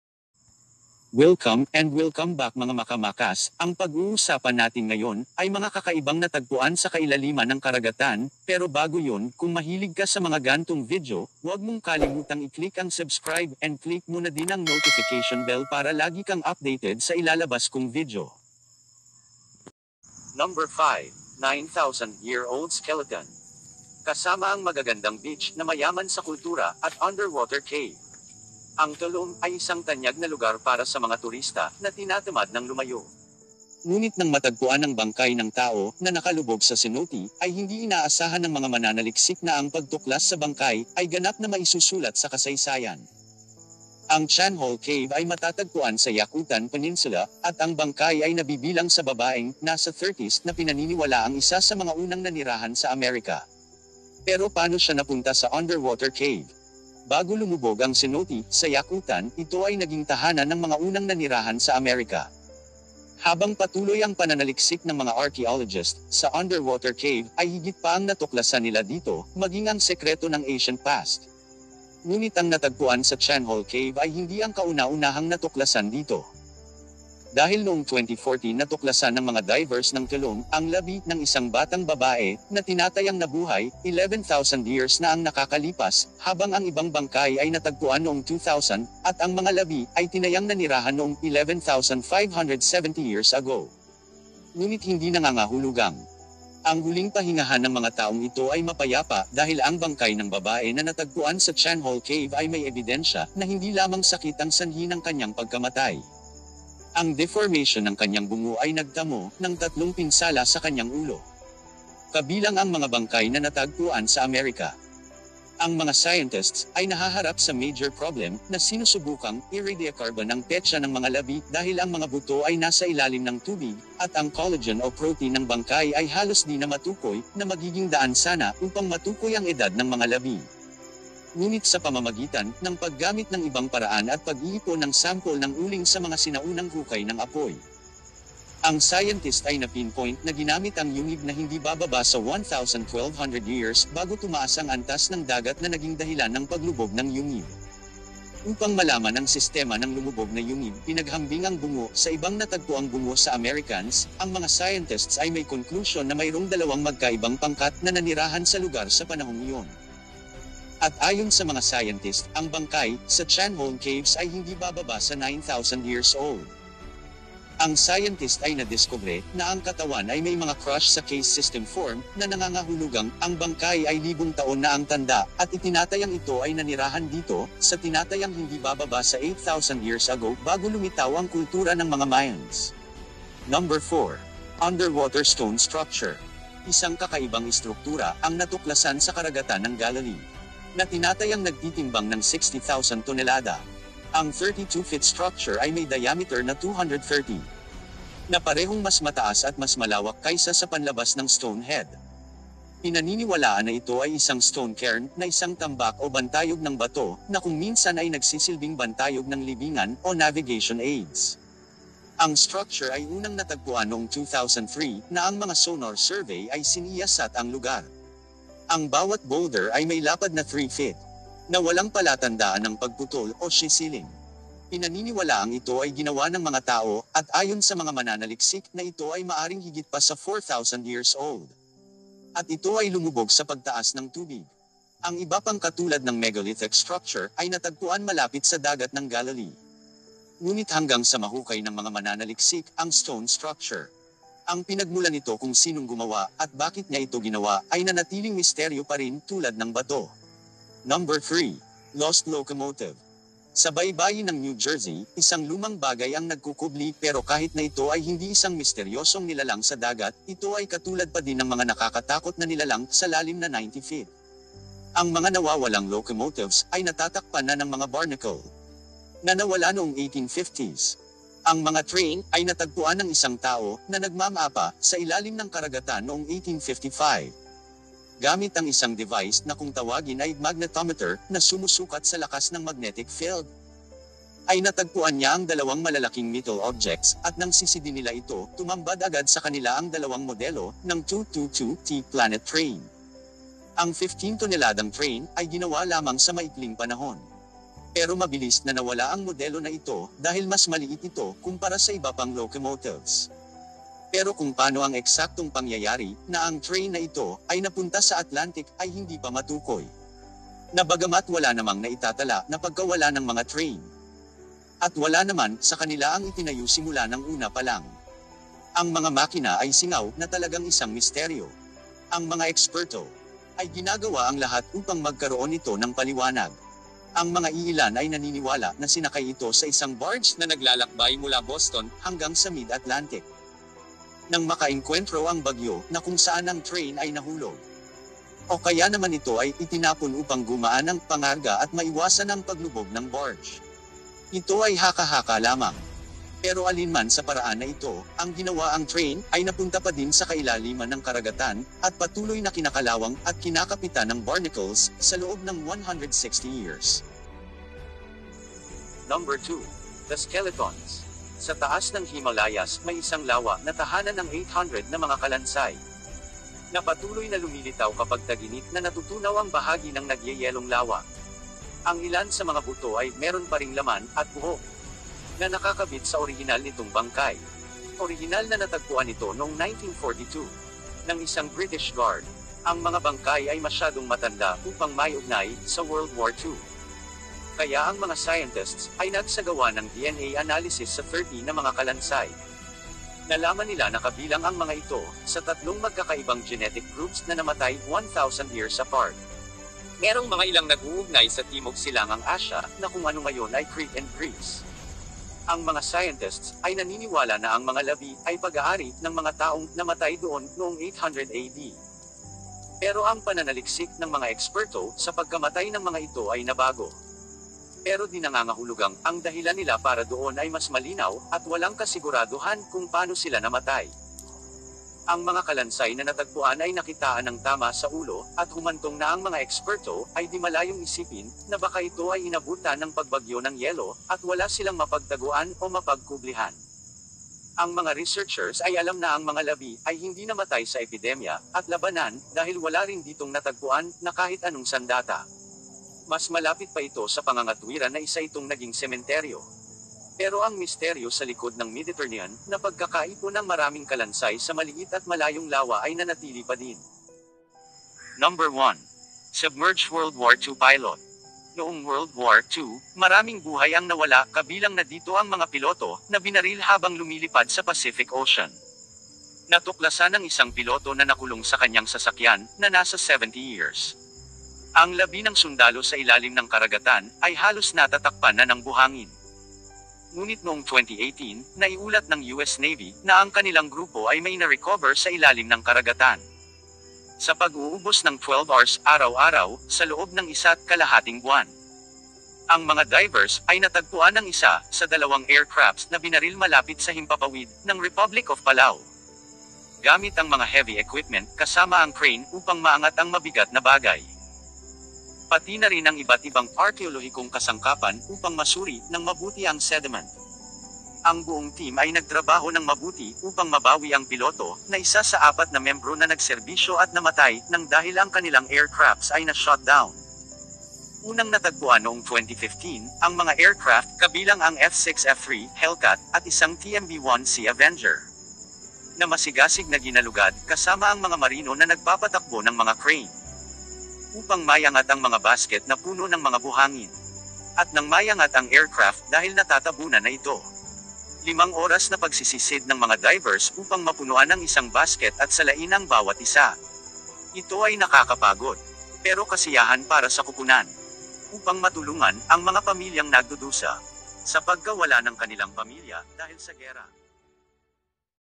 5 KAKAIBANG BAGAY NA NATAGPUAN SA KAILALIMAN NG KARAGATAN | Makas Tv
Mga kakaiban bagay na natagpuan sa kailaliman ng karagatan.